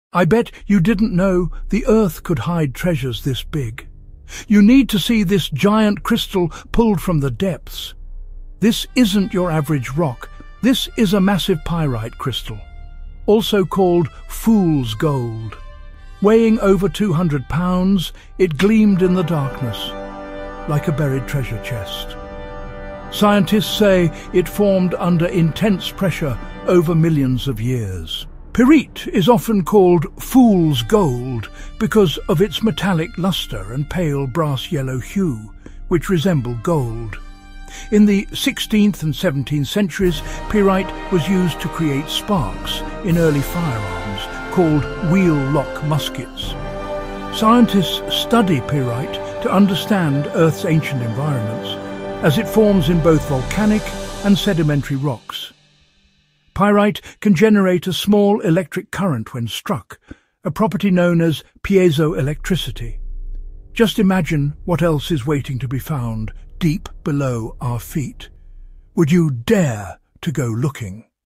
World’s Biggest “Fool’s Gold” Discovery! 😱
Archaeologists and mineral collectors are stunned by this massive pyrite crystal—often called “Fool’s Gold.” But this one’s no joke.
Discovered deep underground, this natural geometric marvel looks like something straight out of science fiction!

📍 Learn what makes pyrite crystals grow in perfect cubes—and why this one might break records!